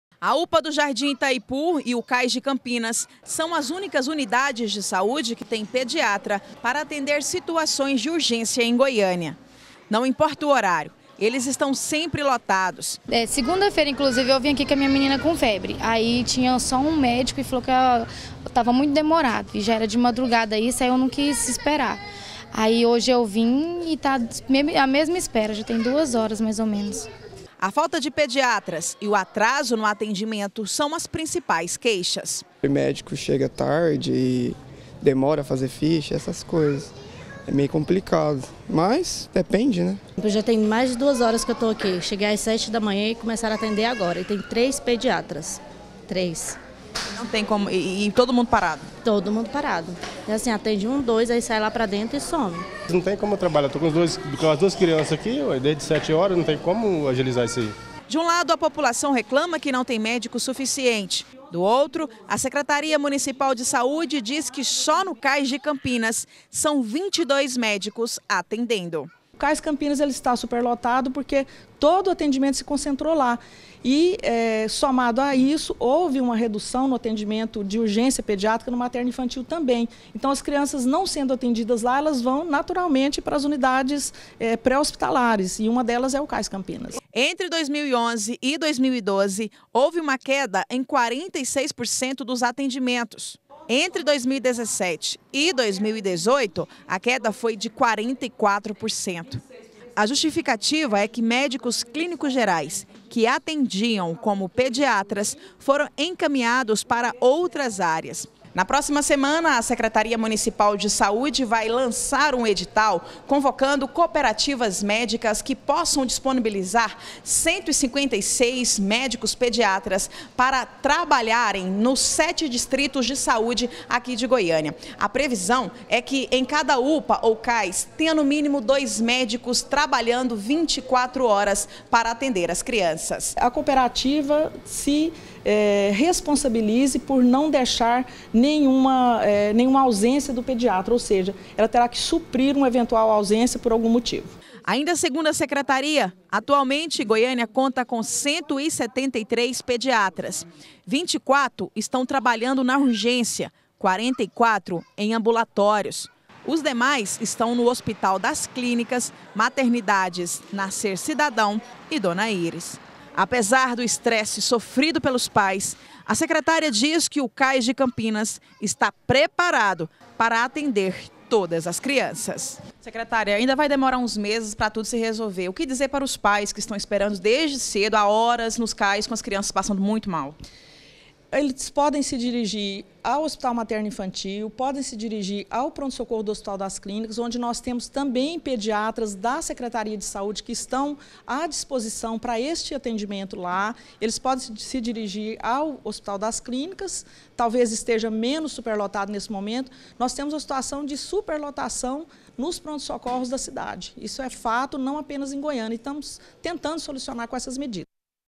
BG - Atendimento pediátrico: prefeitura vai convocar 156 novos profissionais - 04-10-2018
A secretaria municipal de saúde disse que vai publicar um edital na próxima semana para convocar 156 novos pediatras para atender nos sete distritos de saúde de Goiânia.